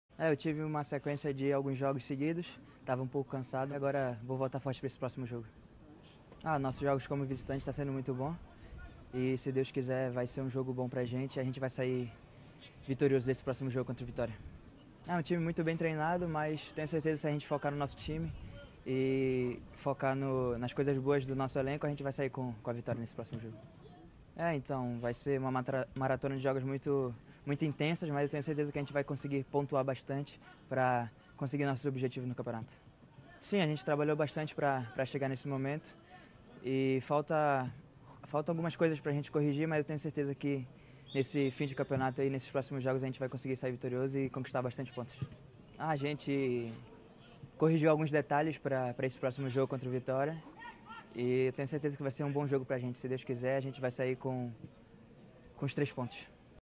Entrevista Airton - Cruzeiro Até o Fim
Veja o que Airton do Cruzeiro disse em entrevista coletiva!